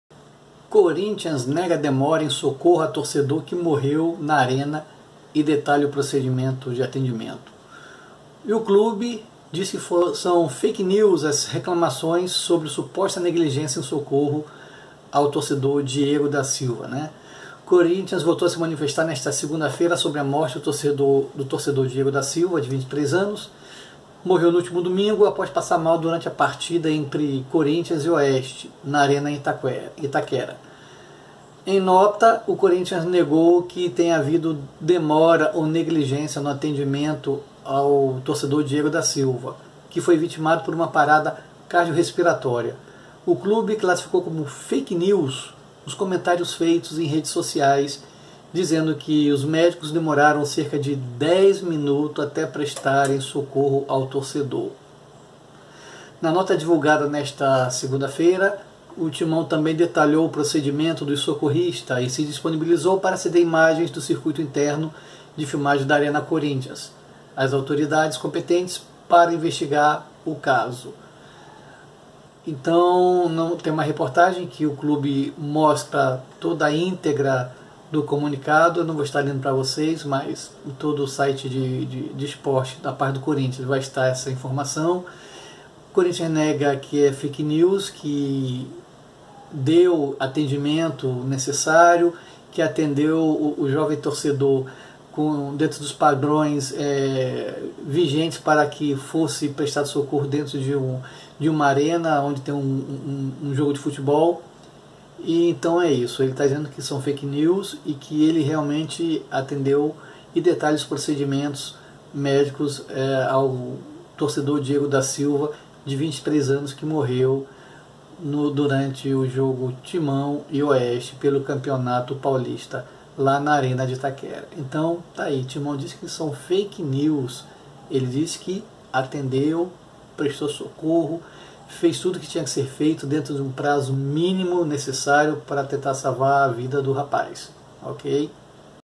CORINTHIANS NEGA DEMORA EM SOCORRO A TORCEDOR QUE MORREU NA ARENA
Corinthians nega demora em socorro a torcedor que morreu na Arena e detalha procedimento.

Clube chama de "fake news" reclamações sobre suposta negligência em socorro a Diego da Silva.

O Corinthians voltou a se manifestar nesta segunda-feira sobre a morte do torcedor Diego da Silva, de 23 anos, que morreu no último domingo após passar mal durante a partida entre Timão e Oeste, na arena de Itaquera.

Em nota, o Corinthians negou que tenha havido demora ou negligência no atendimento a Diego da Silva, que foi vitimado por uma parada cardiorrespiratória.

O clube classificou como "fake news" (termo em inglês para notícia falsa) os comentários feitos em redes sociais dizendo que médicos demoraram cerca de dez minutos até prestarem socorro ao torcedor.

Segundo relatos, os profissionais que estavam na ambulância localizada no gramado se negaram a prestar socorro a Diego, alegando que só poderiam atender a jogadores. Os bombeiros, de acordo com eles, teriam demorado dez minutos até chegarem à arquibancada Norte, onde estava Diego.

Na nota divulga nesta segunda, o Timão também detalhou o procedimento dos socorristas e se disponibilizou para ceder as imagens do circuito interno de filmagem da Arena Corinthians às autoridades competentes pela investigação do caso.

No último domingo, o Corinthians já havia publicado nota oficial lamentando e se solidarizando pelo ocorrido.

Confira abaixo o comunicado emitido pelo Timão nesta segunda-feira:

"O Sport Club Corinthians Paulista vem a público esclarecer boatos e informações inverídicas (fake news) que estão circulando em grupos de mensagens privadas sobre o atendimento ao torcedor Diego da Silva, de 23 anos, que teve uma parada cardiorrespiratória ao final do primeiro tempo do jogo entre Corinthians e Oeste, realizado ontem (17/3) na Arena, e veio a falecer na Unidade de Pronto Atendimento 20 de Agosto para onde foi removido de ambulância.

Segue o histórico da ocorrência:

    16h36m35s - Torcedores alertam o policiamento sobre a ocorrência. O primeiro bombeiro chega ao local 20 segundos depois.
    16h37m45s - Equipe de resgate chega ao local da ocorrência.
    16h38m - Policiamento chega para apoio e isolamento da área.
    16:39m - A Equipe de resgate / bombeiro avaliam as condições do torcedor e iniciam a massagem cardíaca. Torcedor colocado na prancha.
    16:45m - Equipe médica chega no local e assume massagem cardíaca e ventilação mecânica. Começa preparo para remoção do local pela ambulância do campo.
    16:49m - Utilizado desfibrilador para leitura e disparo.
    16:51m - Torcedor é removido para UPA (Unidade de Pronto Atendimento) 20 de agosto, próxima à Arena e o atendimento pela equipe da Arena apoia o atendimento na unidade.

O Sport Club Corinthians Paulista informa que as imagens das câmeras de segurança encontram-se à disposição das autoridades e reitera seu compromisso com a transparência, lamentando o ocorrido que infelizmente resultou na morte prematura de um jovem de 23 anos e volta a se solidarizar com familiares e amigos de Diego neste momento difícil."